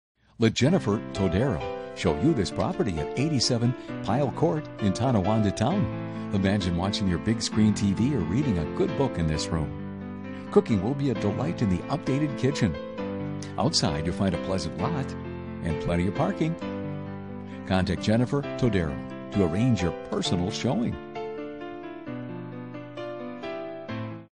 Homes for sale 87 Pyle Court Tonawanda Town NY 14150
MLS Number: B1052540

Enjoy this 3-2 duplex with multi car parking available for both units. Great investment or owner occupy. Unit 1 has a private porch off the dining room with sliding door, updated kitchen, and floors. Bathroom has Jacuzzi tub. Unit 2 has updated flooring and access to large fenced yard. It is close to a park and on a quiet street. Property requires some finishing work: trim, light fixtures, paint, etc. Fence & concrete steps require maintenance.

Let Jennifer Todaro show you this property at 87 Pyle Court in Tonawanda Town. Imagine watching television, reading a good book or listening to music in this pleasant room. Cooking will be a delight in the updated kitchen. Step outside to find a pleasant lot and plenty of parking. Contact Jennifer Todaro to arrange for a personal showing.